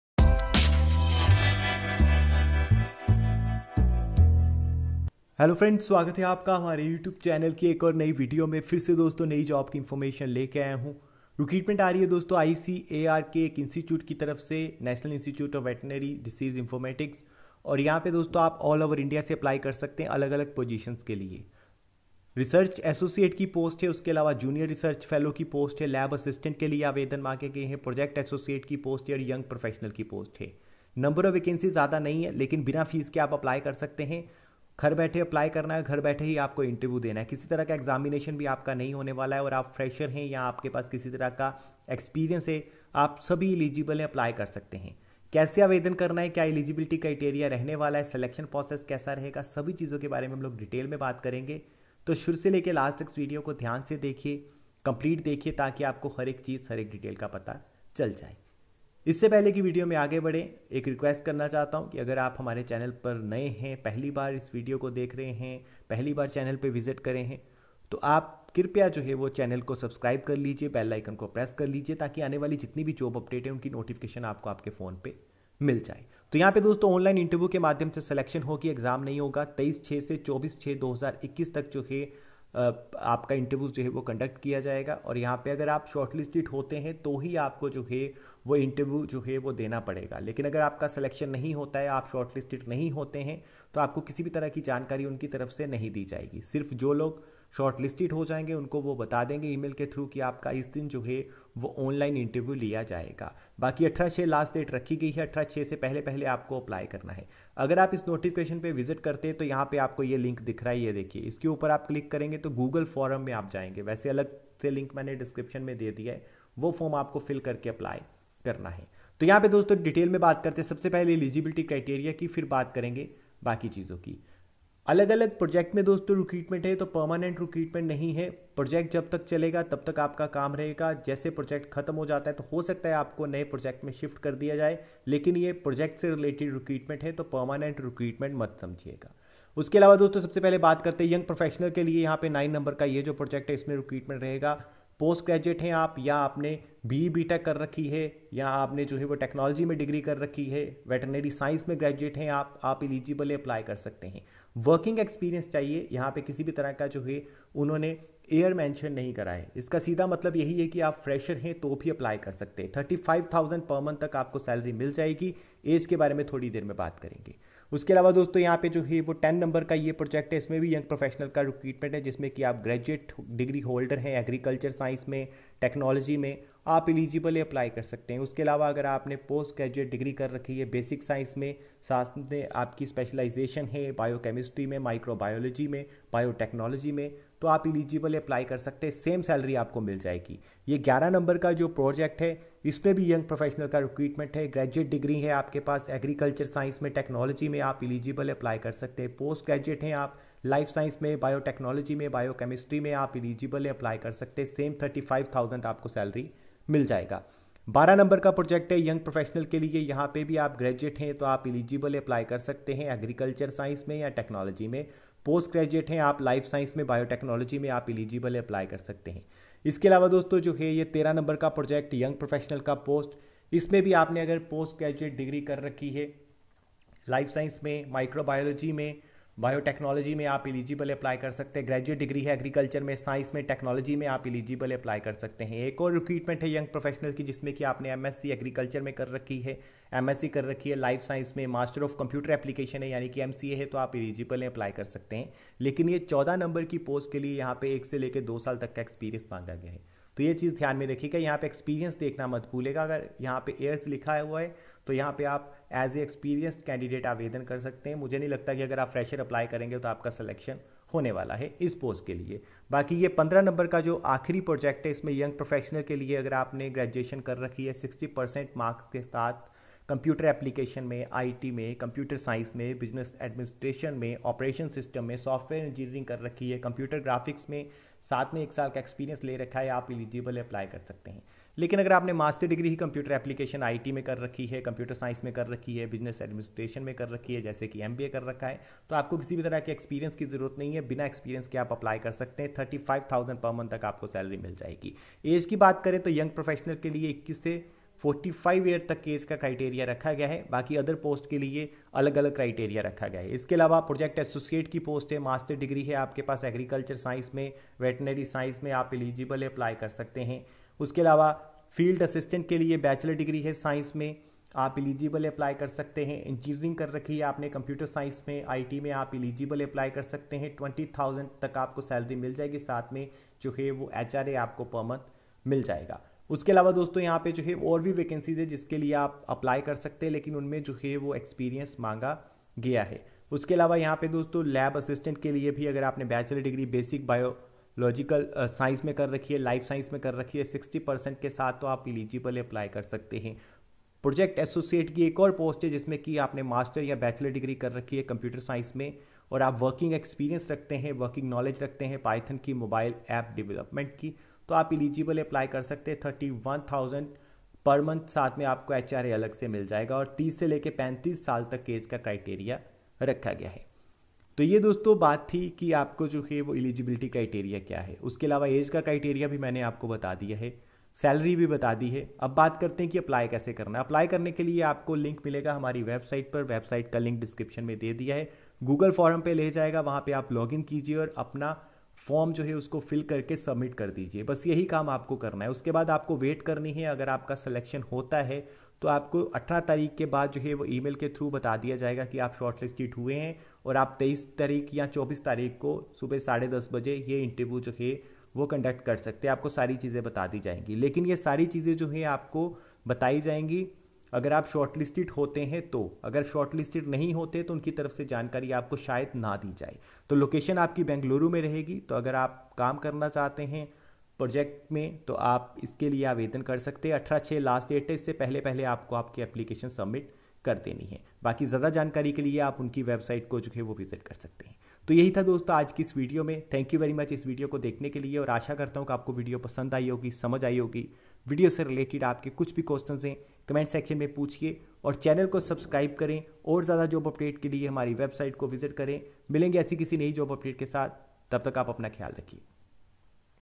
NIVEDI Recruitment 2021 For Young Professional and Other Posts | No Fees | No Exam | Apply Online
ICAR-National Institute of Veterinary Epidemiology and Disease Informatics, Yelahanka, Bengaluru invites online applications from eligible candidates for the posts of  Research Associate – III, One Position of Junior Research Fellow, one position of Lab Assistant, Two Positions of Project Associate -I , One Position of Field Assistant, Two Positions of Senior Research Fellow, Seven Positions of  Young Professional-II, under various projects, Online interview  will be held on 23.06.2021 & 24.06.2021 at 10.30.AM in ICAR-National Institute of Veterinary Epidemiology and Disease Informatics, Yelahanka, Bengaluru-560064 and all the positions are co-terminus with the respective projects. Candidates are required to fill  application form by using this link Given Below.